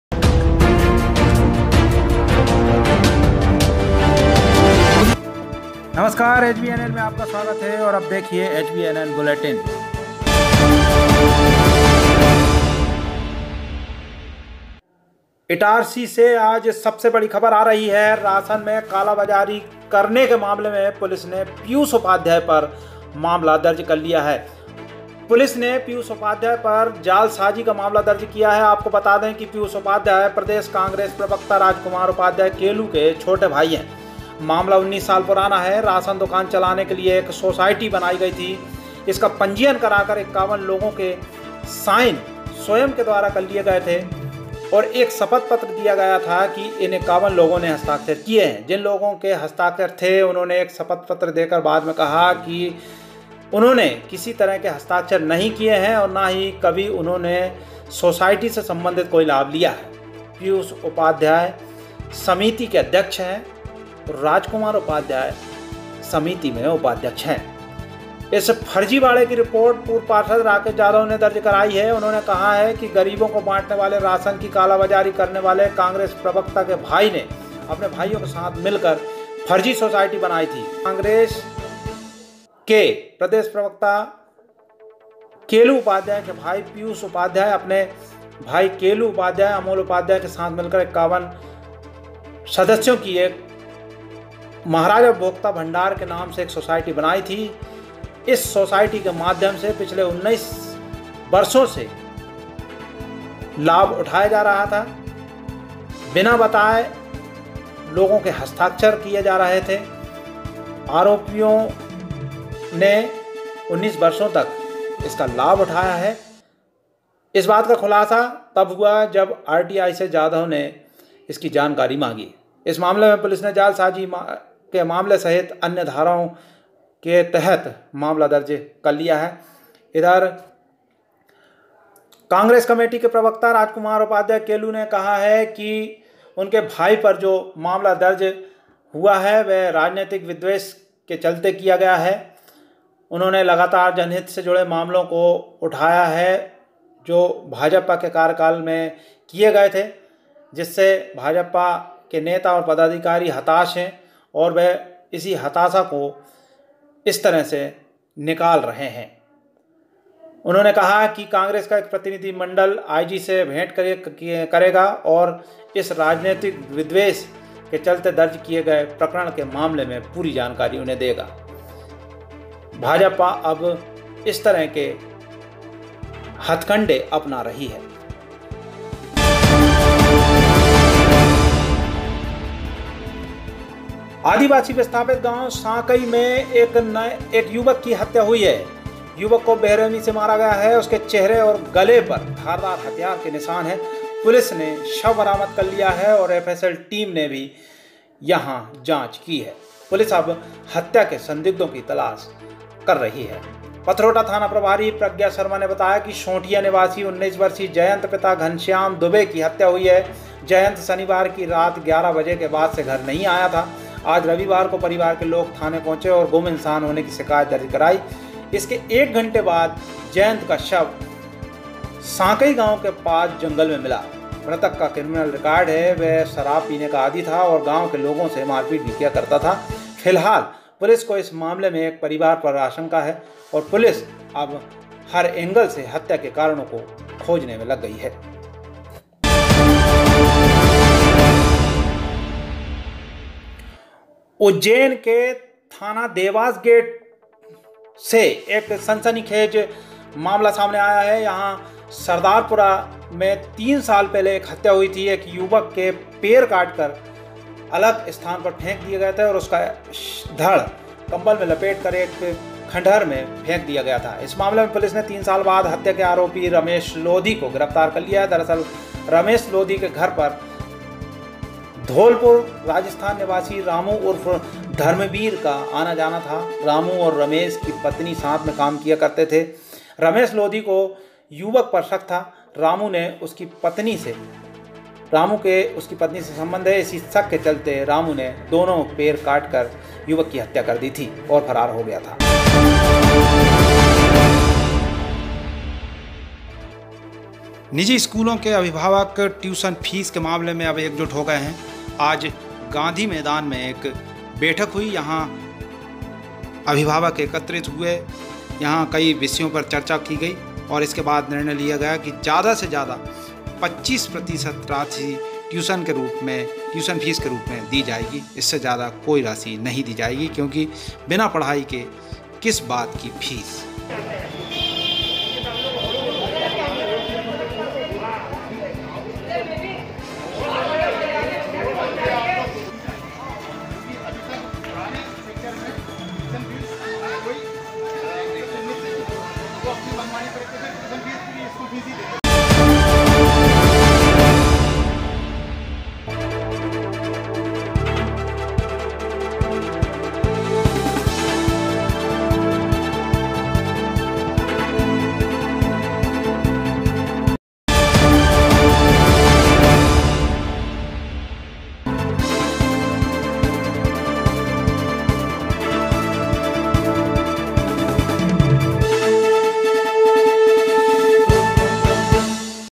सबसे बड़ी खबर- प्रदेश कांग्रेस प्रवक्ता के छोटे भाई पर अपराध दर्ज
कांग्रेस प्रवक्ता राजकुमार उपाध्याय केलू के छोटे भाई पीयूष उपाध्याय पर जालसाजी का मामला दर्ज, पूर्व पार्षद राकेश जाधव ने दर्ज कराई रिपोर्ट, राशन दुकान के लिए महाराजा सोसायटी के 51 सदस्यों के साइन स्वयं ने कर लिए थे, युवक के जंगल में मिली लाश, मैं और सिर पर धारदार हथियार से हमले, 3 साल पहले उज्जैन में पैर काटकर की गई थी युवक की हत्या,
अभिभावकों की बैठक 25% से ज्यादा फीस नहीं करेंगे जमा
Itarsi, pipariya, siwni malwa, hoshangabad, local news, crime news, crime news,